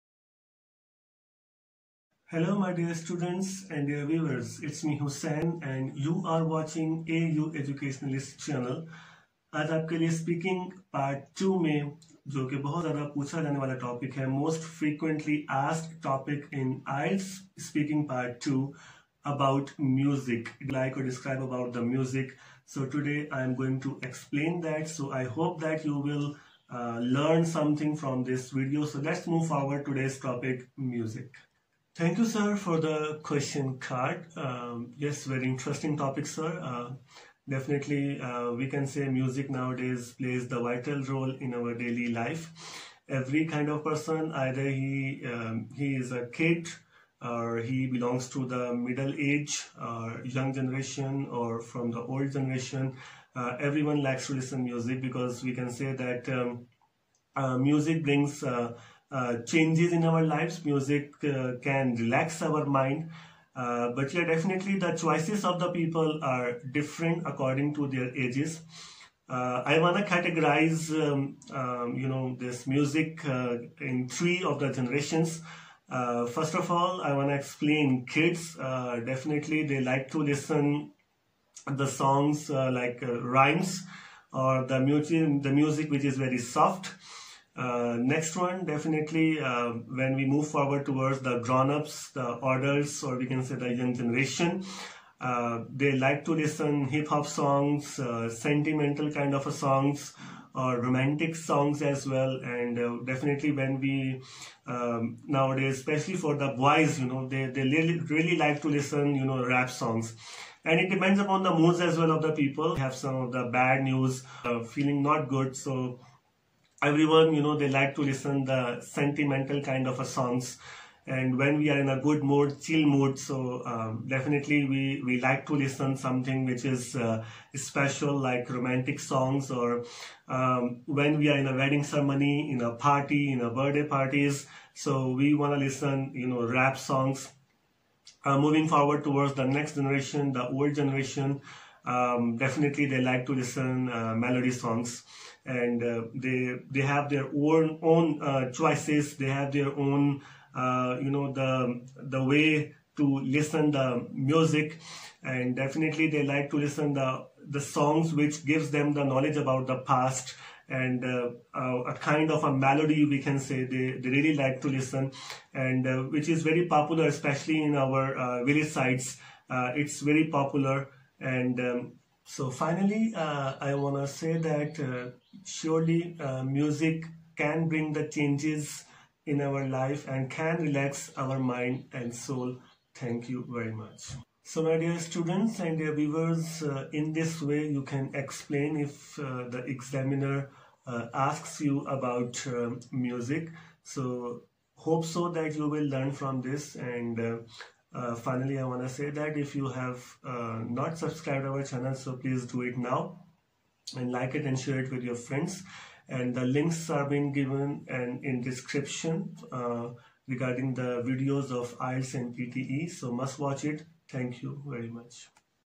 IELTS speaking part-2 MUSIC
This video will explain the viewers that how a student can explain about music in front of examiner in speaking part-2 in IELTS speaking section. The demo has been provided to the students to learn how a topic can be answered in speaking part-2.